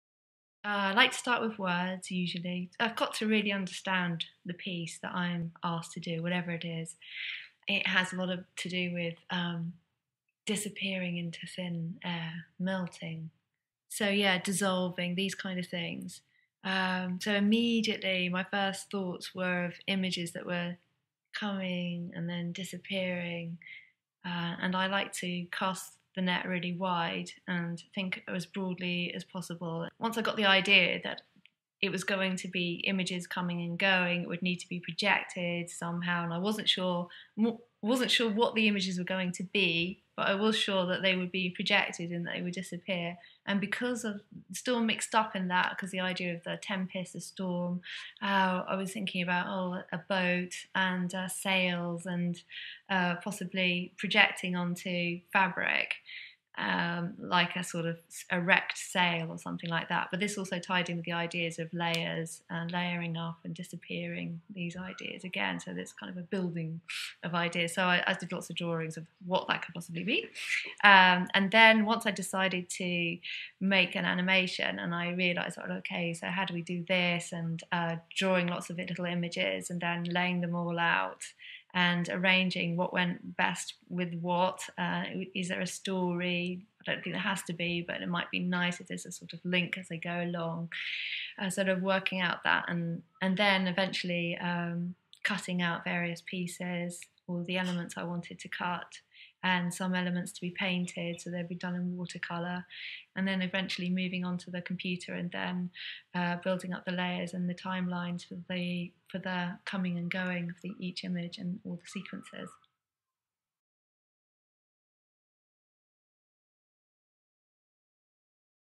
Well Said! Favourite Shakespearean quotes - Margaret Atwood
Well Said is a visual feast of Shakespeare’s most memorable and inspiring lines. Writers, actors and poets have selected their favourite quotes to be transformed into surprising and beautiful artworks.

Author Margaret Atwood whose next novel, HAG-SEED, sees her retell the story of Shakespeare's The Tempest as part of the Hogarth Shakespeare project saw her quote transformed by artist Lara Harwood.

How did the themes of The Tempest inform not only the images but the way in which they would be viewed?

With thanks to Lara Harwood and the RSC.

You can find out more about the free exhibition Well Said